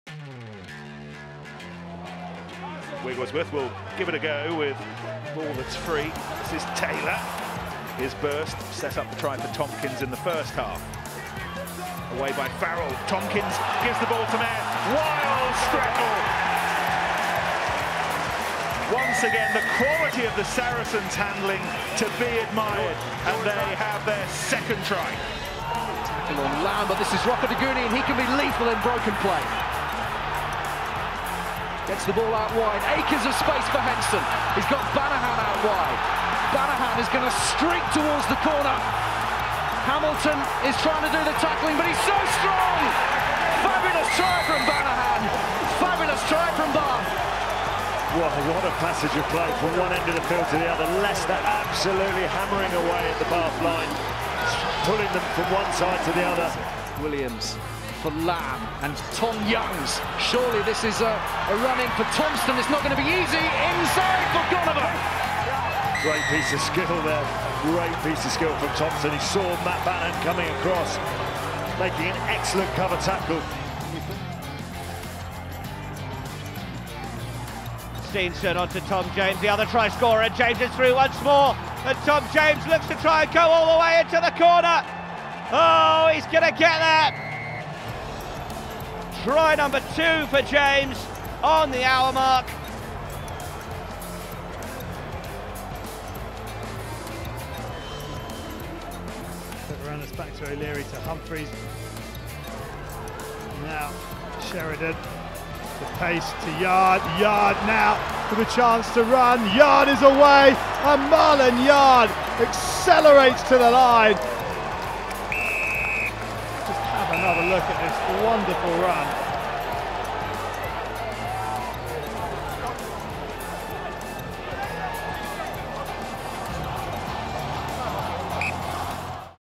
Try Of The Week - Round 2 - Strettle, Banahan, Goneva, James & Yarde
Vote for the best try from round 2 of the Aviva Premiership.

1. David Strettle - Saracens vs Gloucester
2. Matt Banahan - Bath vs Leicester Tigers
3. Niki Goneva - Bath vs Leicester Tigers
4. Tom James - Exeter Chiefs vs London Wasps
5. Marland Yarde - Worcester Warriors vs London Irish